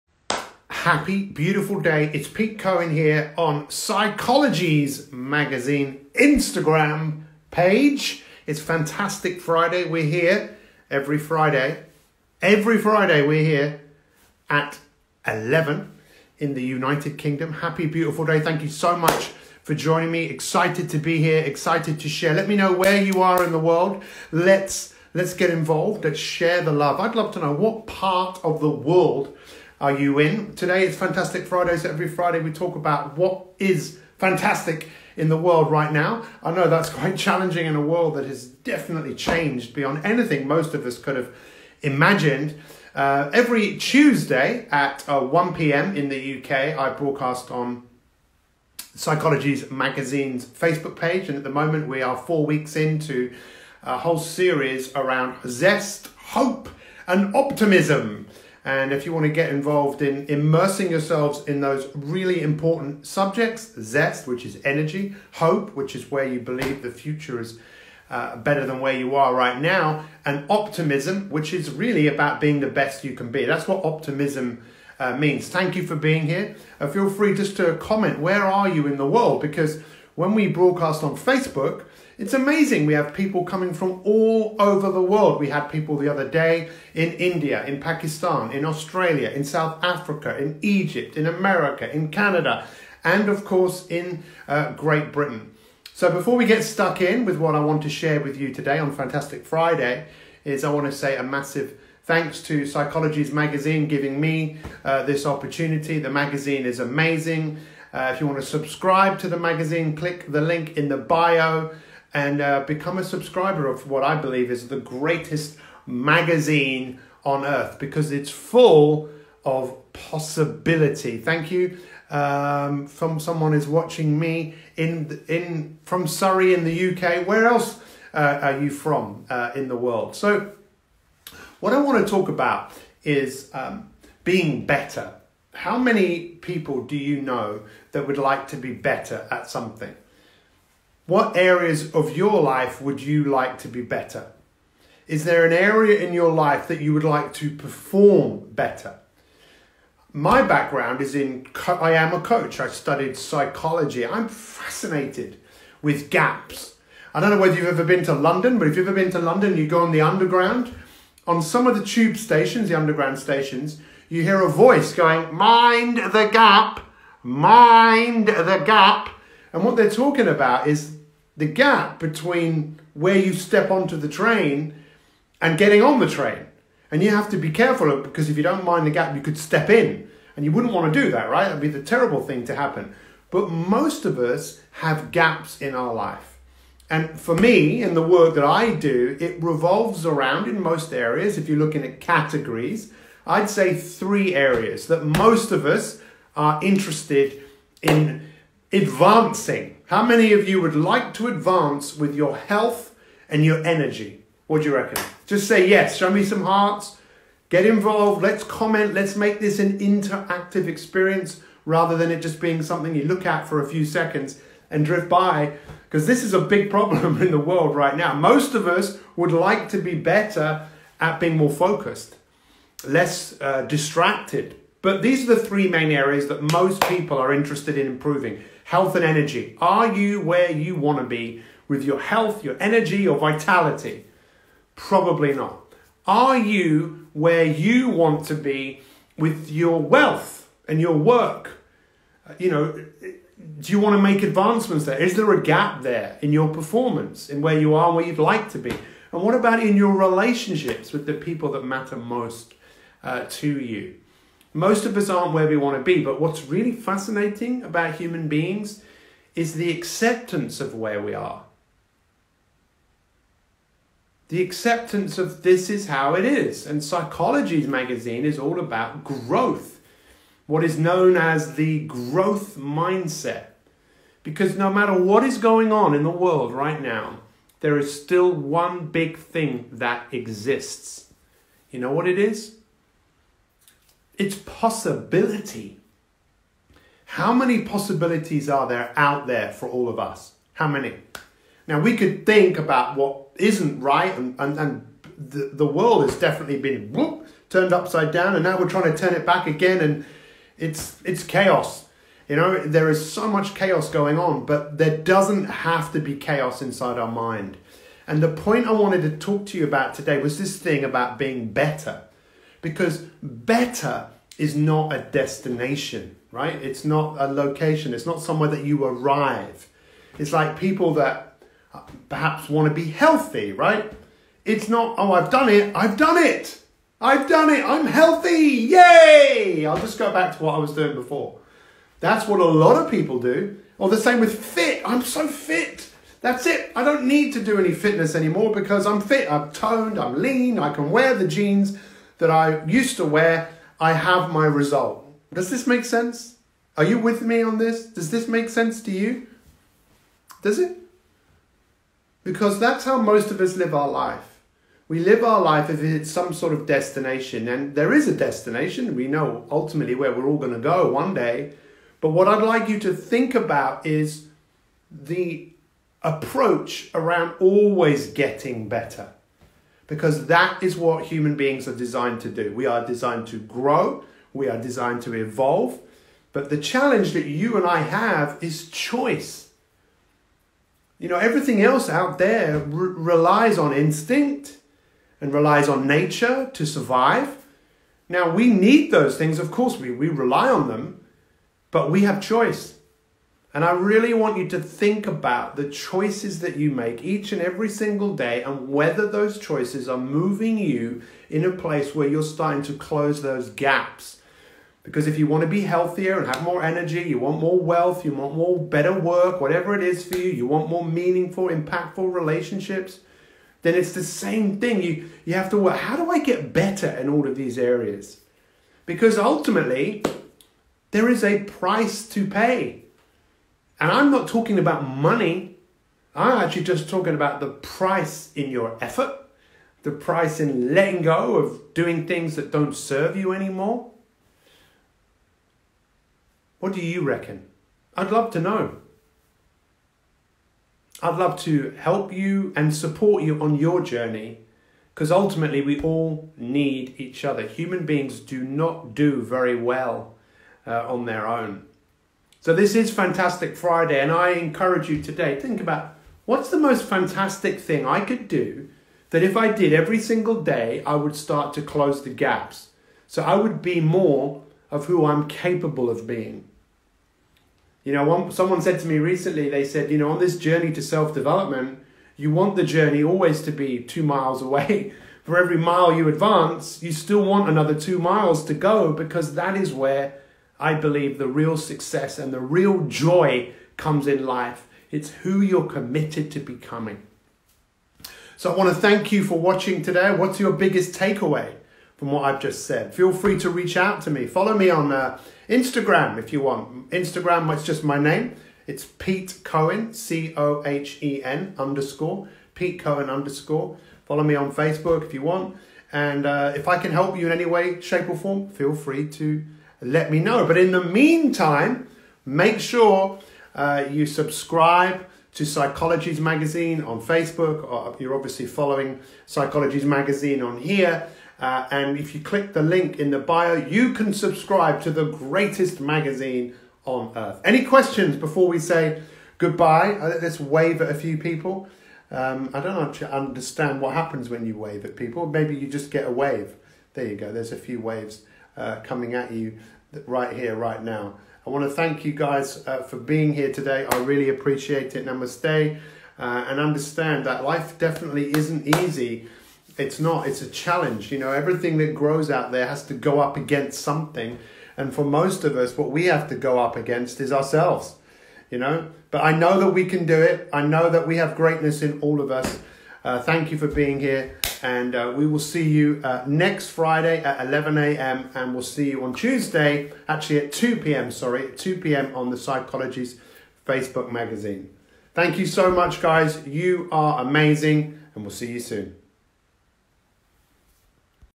Fantastic Friday with coach and motivational speaker Peter Cohen
It’s a fantastic Friday but what would make it even more fantastic it’s us deciding to get better.

Better is not a destination better is a process that never ever ends.

What would your life be like if you decided to advance and get better in the areas of your life that really matter.

For most people these revolve around her health and energy, their relationships and their work. What are ways that you could start close the gaps between where you are and where you want to be? @petecohen_